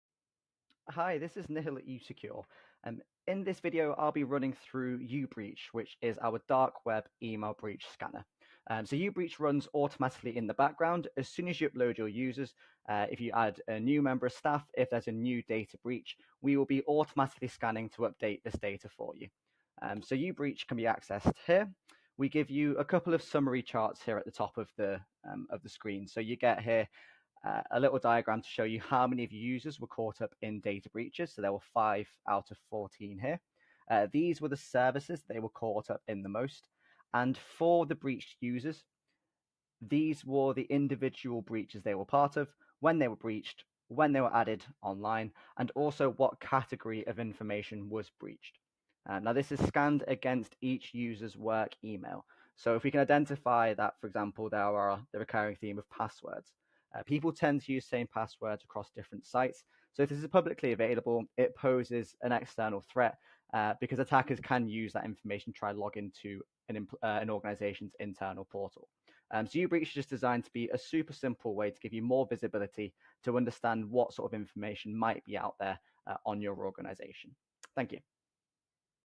usecure - Dark Web Breach Monitoring
Learn how usecure's dark web monitoring tool, uBreach, continuously scans data dumps, paste sites and more for breached employee credentials.

These credentials could be used to launch hyper-targeted phishing attacks against your business.

Start detecting your employees' exposed usernames, passwords and more. For more information around usecure, contact our sales team.